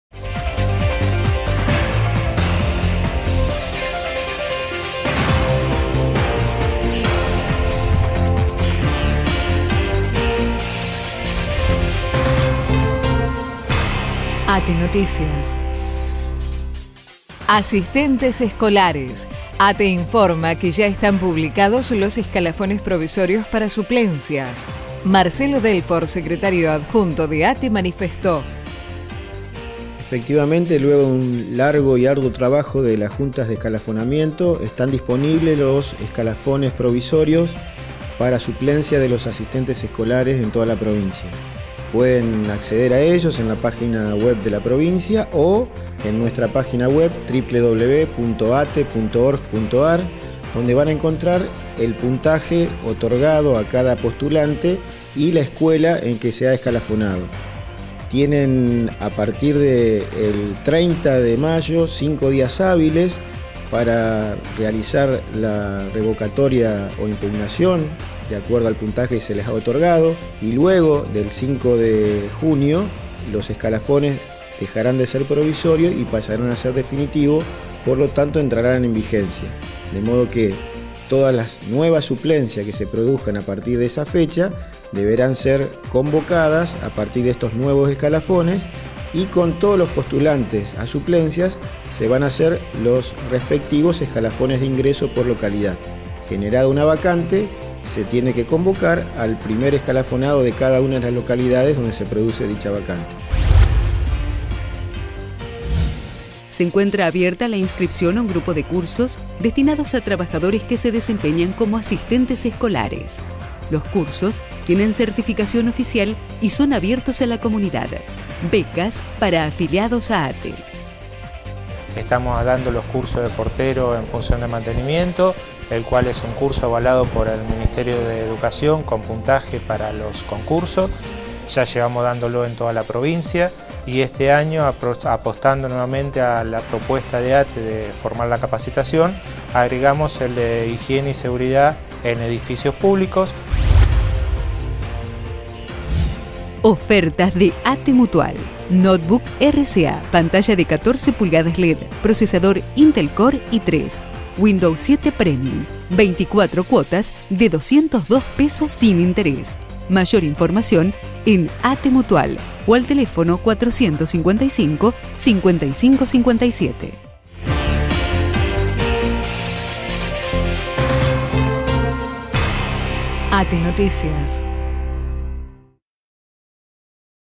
MICRO ATE 207
Este micro fue emitido el 27 de Mayo de 2013 en Noti13 1º Edición.
Contiene: Hasta el 29 de mayo estarán publicados los escalafones provisorios (Marcelo Delfor); Formación 2013: Cursos de mantenimiento (Ignacio Vallejos,Jorge Bianchini); Ofertas ATE Mutual: Notebook RCA 14 pulgadas led.